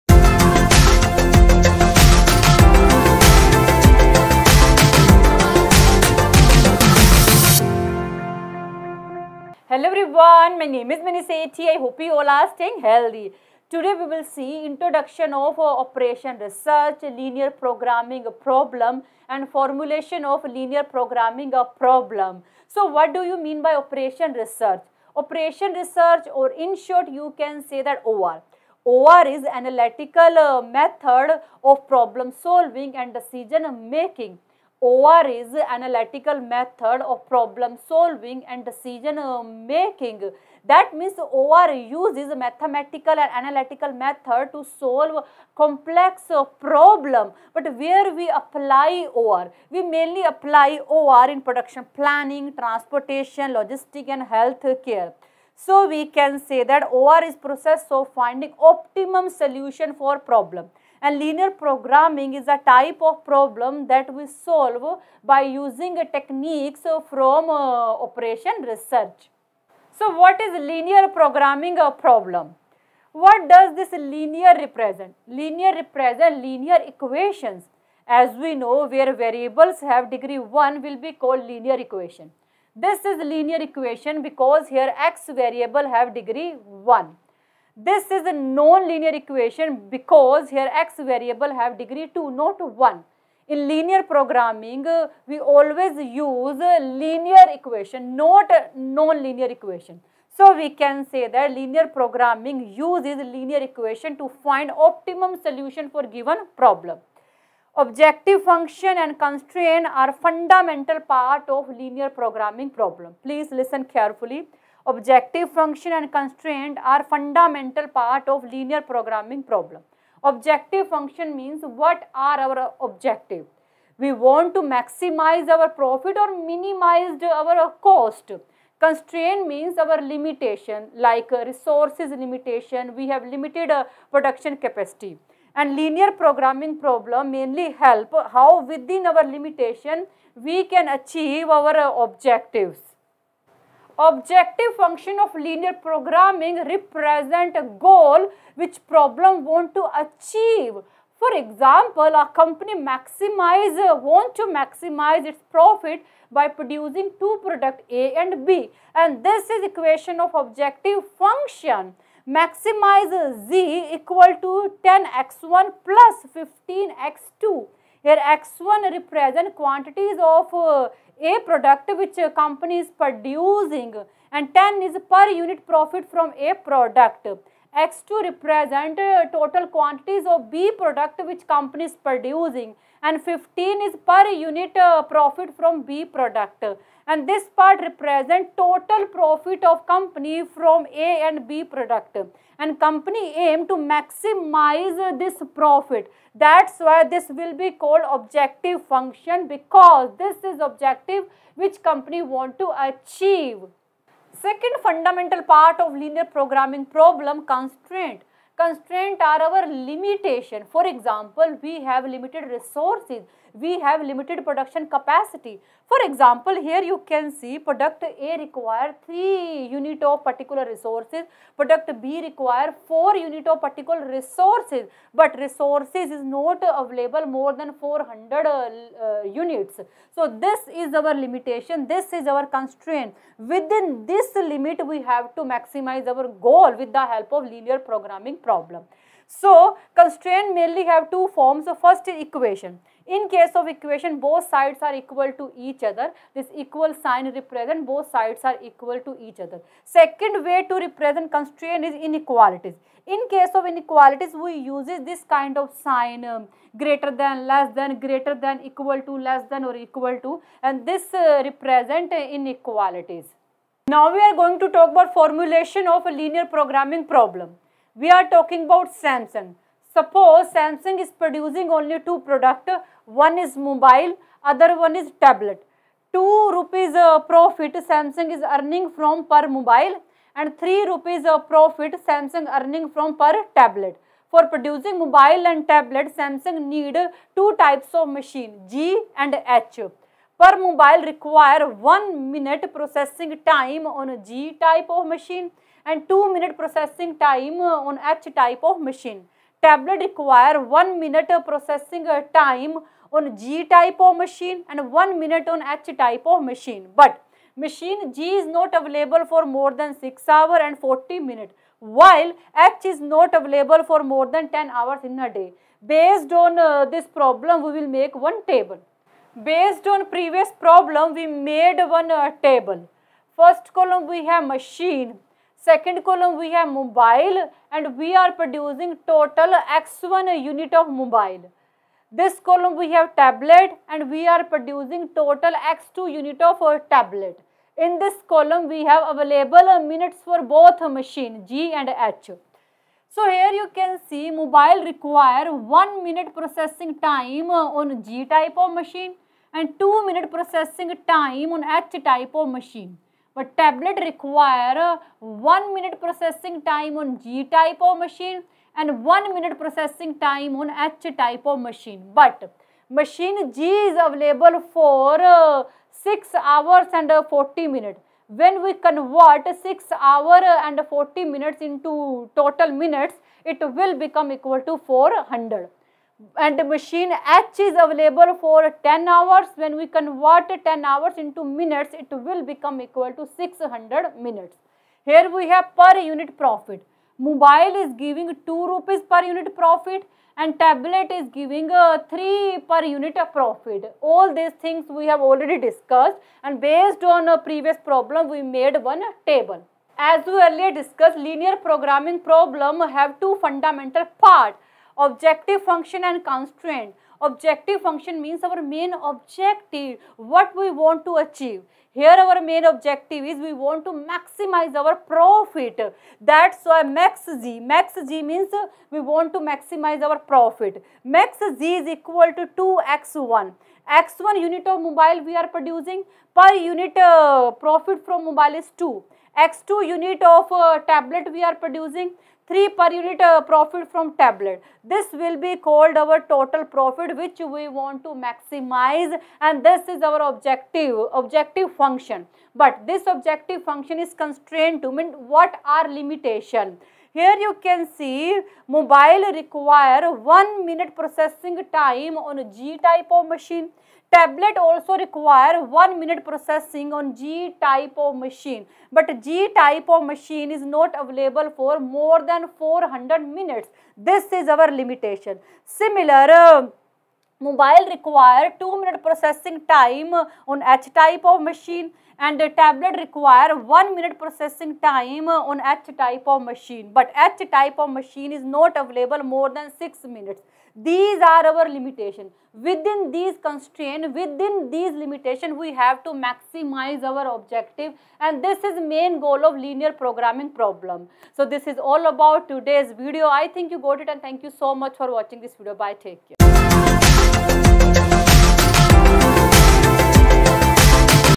Introduction of Operation Research, Linear Programming Problem, Formulation of Linear Programming
This video describes about Introduction of Operation Research, Linear Programming Problem, Formulation of Linear Programming Problem